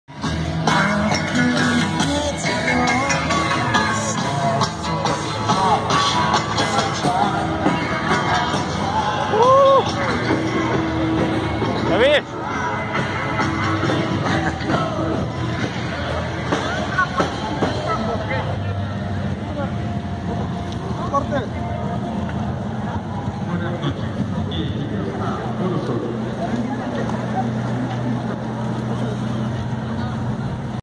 GUNS N' ROSES LIVE MÉXICO 2016 Llegando al Foro Sol
Momento en el que mi hermano y yo llegamos a el foro sol...